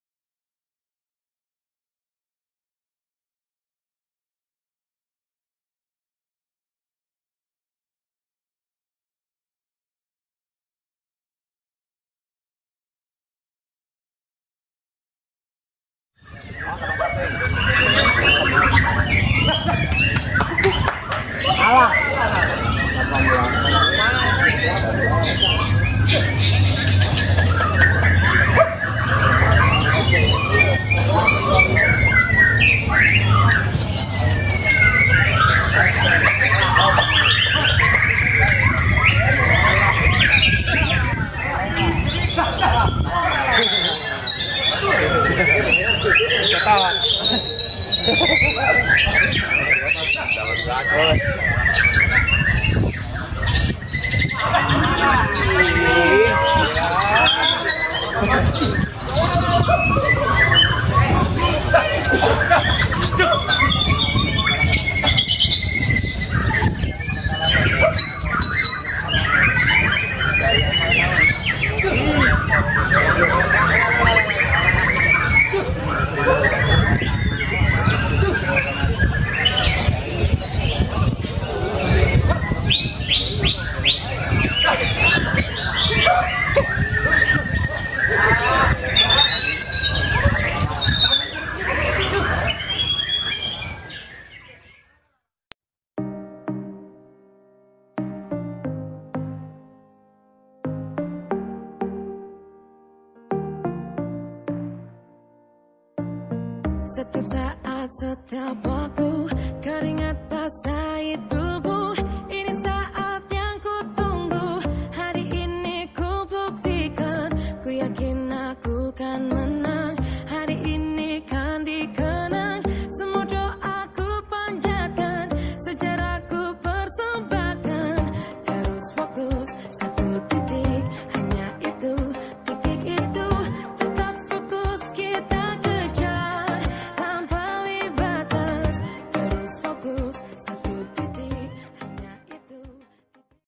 Kontes Burung Murai Batu Ring APBN BTC Asrama Denjasa Ang
Video Ini berisikan kontes burung Murai Batu khusus ring APBN(Asosiasi Penangkar Burung Nusantara), yang diselenggarakan di BTC Asrama Denjasa Ang Kiaracondong Bandung pada hari Sabtu, 8 Februari 2020